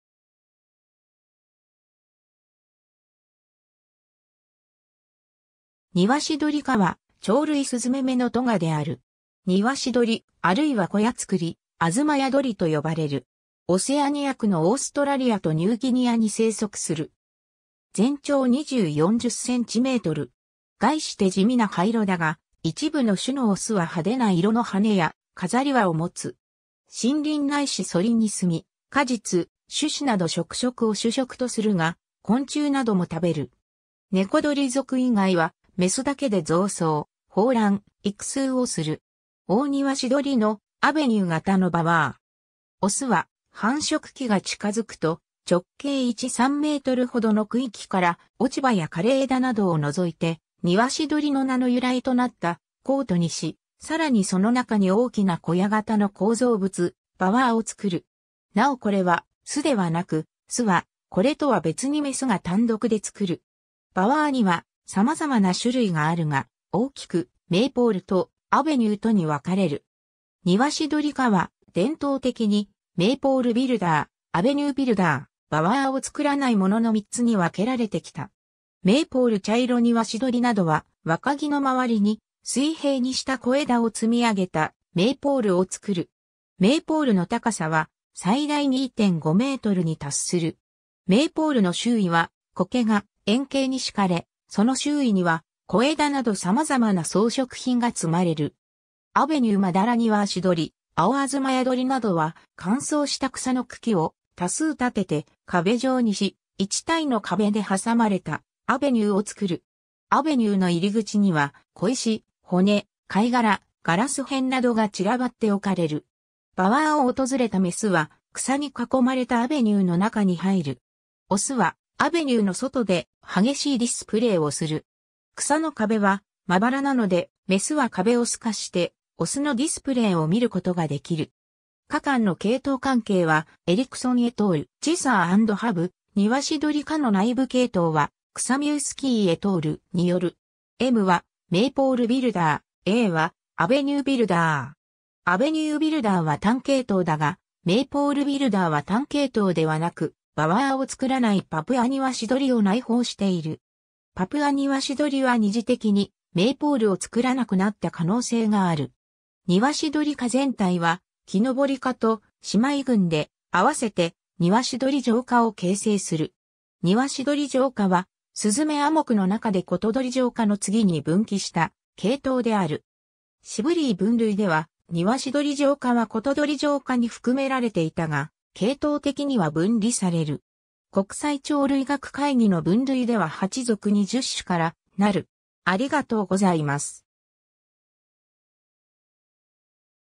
ニワシドリ科
ニワシドリ科 (ニワシドリか、学名 Ptilonorhynchidae) は、鳥類スズメ目の科である。
ニワシドリ、あるいはコヤツクリ、アズマヤドリと呼ばれる。
 オセアニア区のオーストラリアとニューギニアに生息する。
 全長20–40cm。
 概して地味な羽色だが、一部の種の雄は派手な色の羽や飾り羽を持つ。
 森林ないし疎林に住み、果実・種子など植食を主食とするが、昆虫なども食べる。
 ネコドリ属以外は、雌だけで造巣・抱卵・育雛をする。
 オオニワシドリの「アベニュー」型のバワー。
 雄は繁殖期が近づくと、直径1–3mほどの区域から落ち葉や枯れ枝などを除いて、ニワシドリの名の由来となった「コート (court、庭)」にし、さらにその中に大きな小屋型の構造物「バワー (bower、あずまや)」を作る。
なおこれは巣ではなく、巣はこれとは別に雌が単独で作る。
 バワーにはさまざまな種類があるが、大きく「メイポール (maypole)」（「スピア (spire)」を含む）と「アベニュー (avenue)」とに分かれる。
ニワシドリ科は伝統的に、メイポールビルダー、アベニュービルダー、バワーを作らないものの3つに分けられてきた。
 メイポール チャイロニワシドリなどは、若木の周りに、水平にした小枝を積み上げた「メイポール」を作る。
メイポールの高さは最大2.5メートルに達する。
 メイポールの周囲はコケが円型に敷かれ、その周囲には小枝などさまざまな装飾品が積まれる。
 アベニュー マダラニワシドリ、アオアズマヤドリなどは、乾燥した草の茎を多数立てて壁状にし、一対の壁で挟まれた「アベニュー」（道）を作る。
アベニューの入り口には、小石、骨、貝殻、ガラス片など（どのようなものが好まれるかは種によって異なる）が散らばって置かれる。
 バワーを訪れた雌は、草に囲まれたアベニューの中に入る。
雄はアベニューの外で、激しいディスプレイをする。
草の壁はまばらなので、雌は壁を透かして雄のディスプレイを見ることができる。
 科間の系統関係は Ericson et al. (2002); Chesser & Have (2007)、ニワシドリ科の内部系統は Kusmierski et al. (1997)による。
[M] はメイポールビルダー、[A] はアベニュービルダー。
 アベニュービルダーは単系統だが、メイポールビルダーは単系統ではなく、バワーを作らないパプアニワシドリを内包している。
パプアニワシドリは二次的にメイポールを作らなくなった可能性がある。
 ニワシドリ科全体はキノボリ科と姉妹群で、合わせてニワシドリ上科を形成する。
ニワシドリ上科は、スズメ亜目の中でコトドリ上科の次に分岐した系統である。
Sibley分類では、ニワシドリ上科はコトドリ上科に含められていたが、系統的には分離される。
 国際鳥類学会議 (IOC) の分類では8属20種からなる。